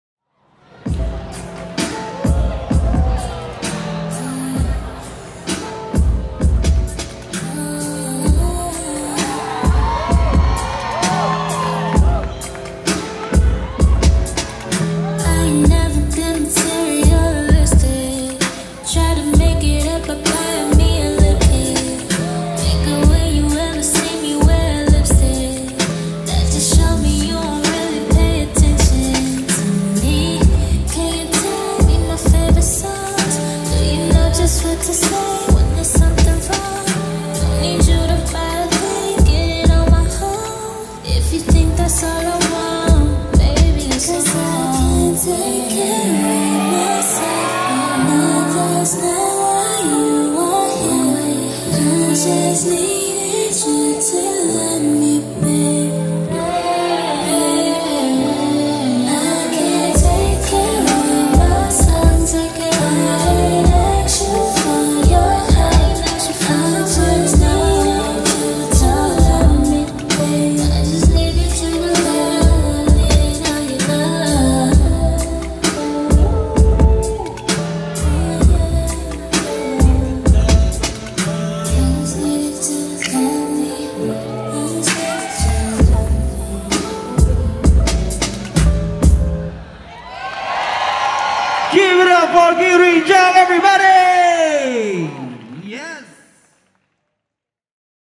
Girin Jang (KR) | COTY Judge Showcase | Eat D Beat 2019 | RPProds
Eat D Beat 2019
This event is presented by
Rocket Crew & Bridge Dance Academy

DJ: Gandhi & J-Beats
Emcee: Raben

Filmed by Richard Prayoga & Desirae Tan
Edited by Richard Prayoga
Thumbnail photo by Edwin Prasetyo